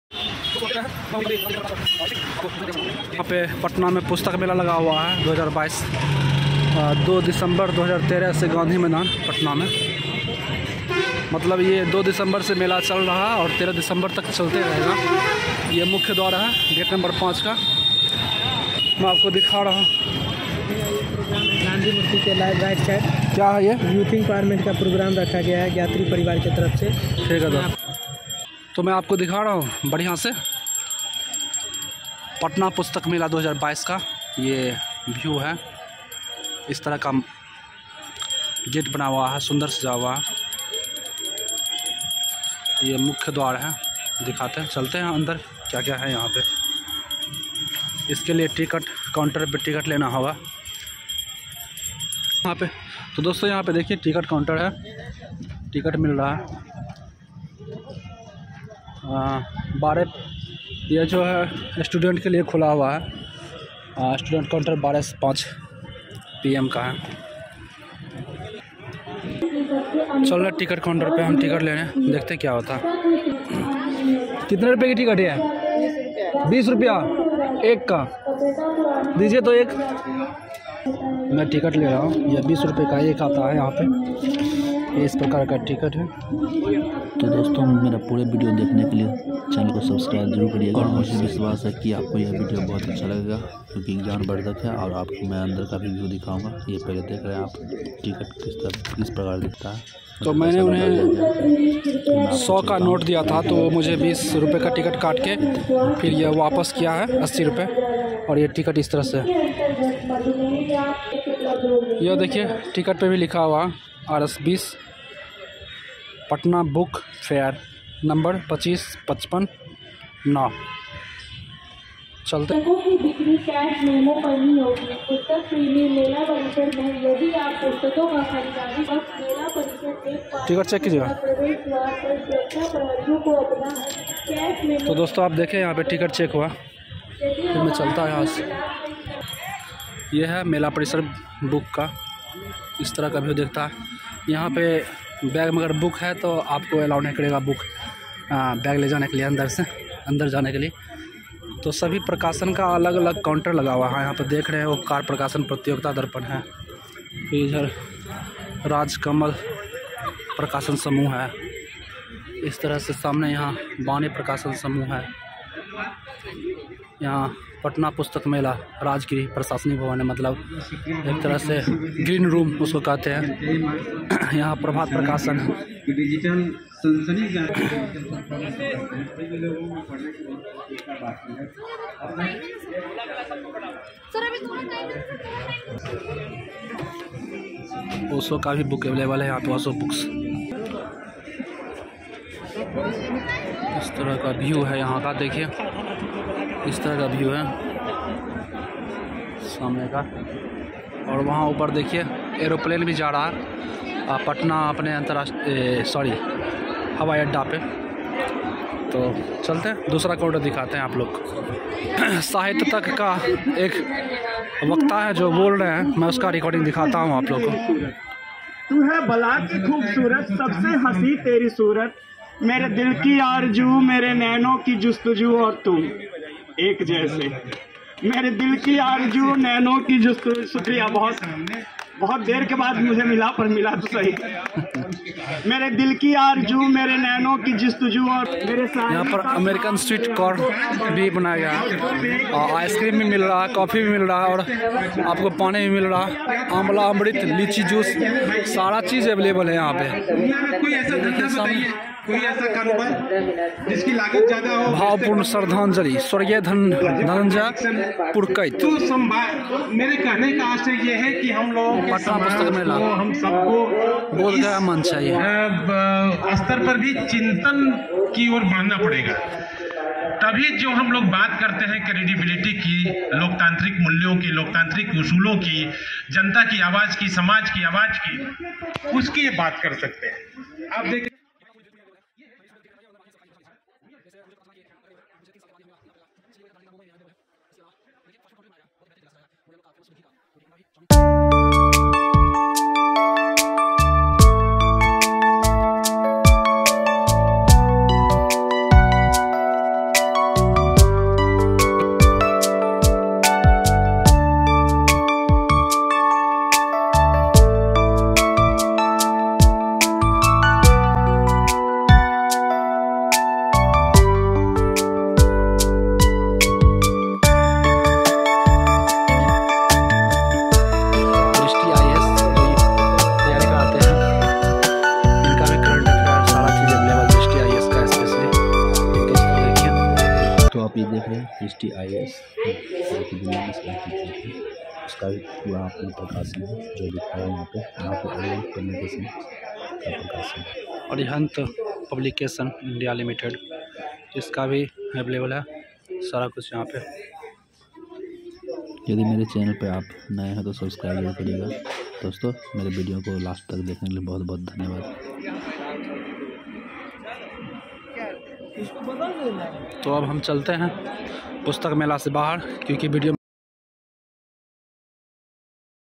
Patna Book Fair 2022@prvlogs94 पुस्तक मेला पटनाvlogs पटना के गांधी मैदान में पुस्तक मेला
Patna Book Fair 2022#youtube पुस्तक मेला पटना।@prvlogs94  पटना के गांधी मैदान में पुस्तक मेला

✍️मुझे 🛑फॉलो करने के लिए नीचे दिए गए लिंक पर क्लिक कर के संपर्क कर सकते हैं।